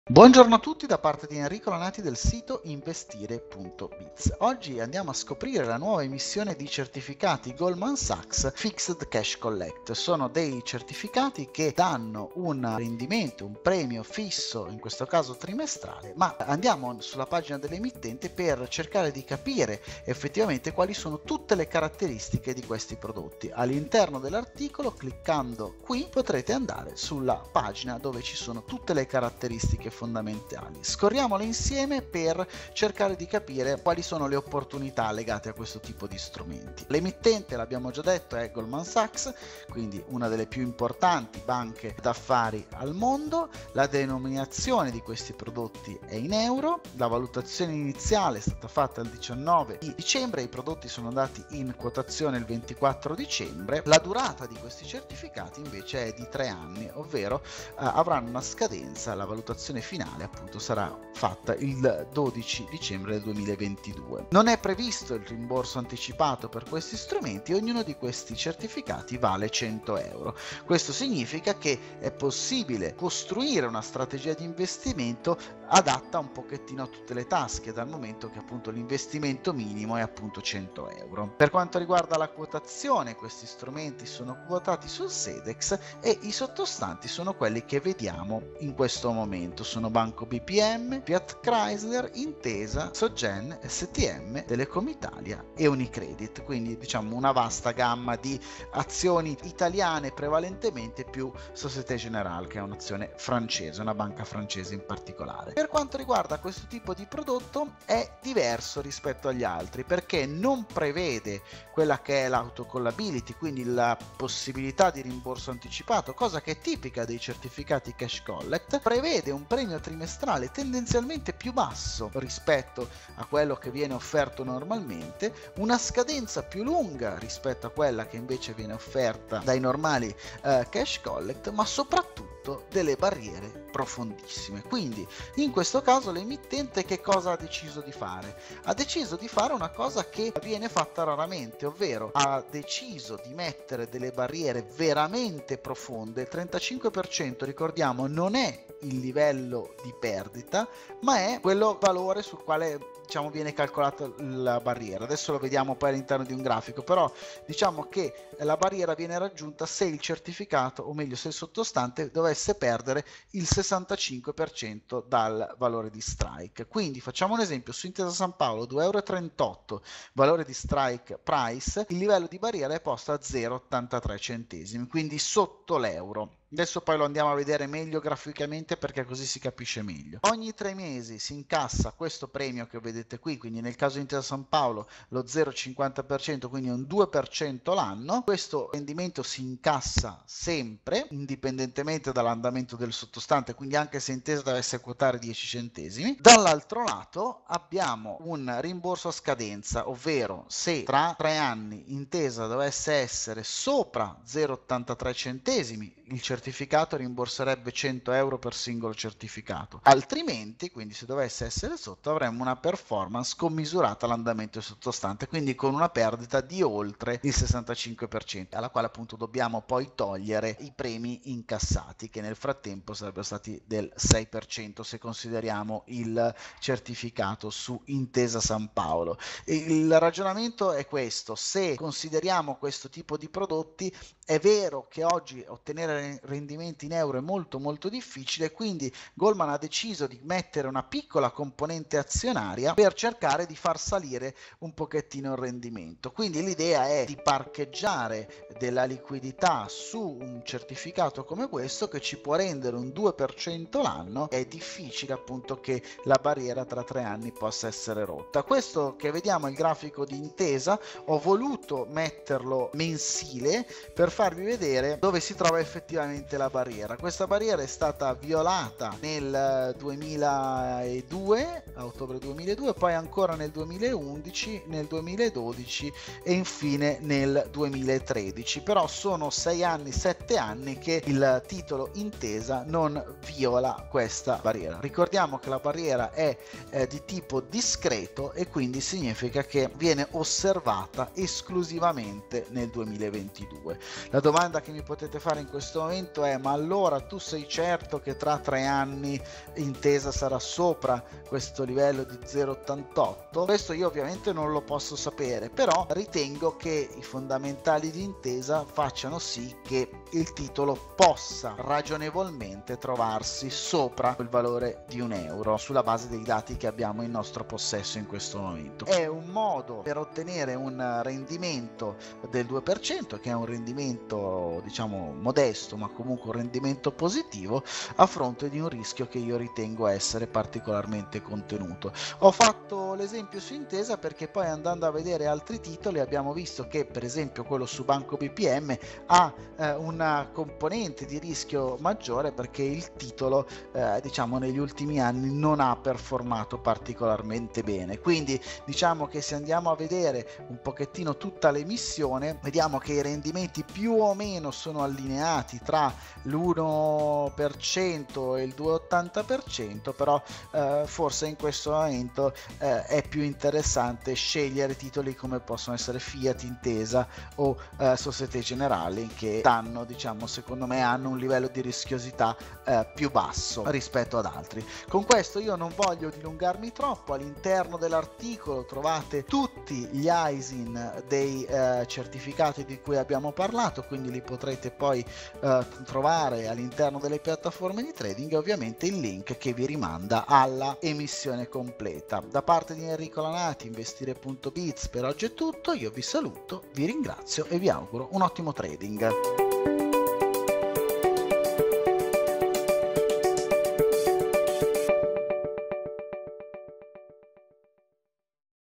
CASH COLLECT su BANCA INTESA di Goldman Sachs
Goldman Sachs ha appena emesso una nuova gamma di Fixed Cash Collect.
I certificati quotati sul SeDeX hanno un prezzo di emissione di 100 Euro.